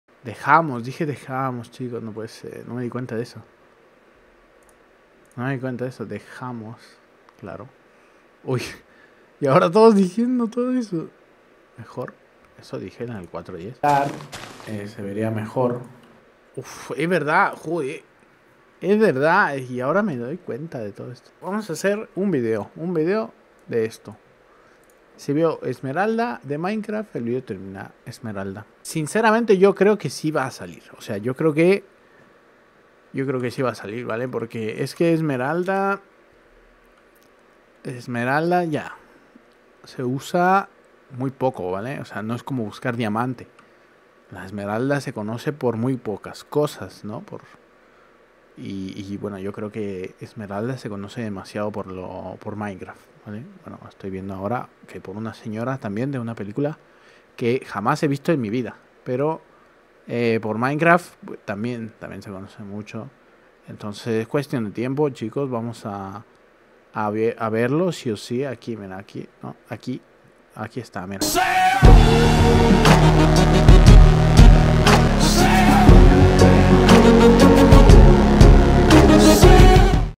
SI VEO ESMERALDA DE MINECRAFT EL VIDEO TERMINA - Esmeralda
Comenzamos con un nuevo vídeo, donde si llego a ver una esmeralda de minecraft en resultado de google imagenes " Esmeralda ", por muy poco que sea, una sola cosa de cualquier tipo y cantidad, el vídeo termina inmediatamente.

APOYO:
UNETE como seguidor y consigue ventajas asì como insignias: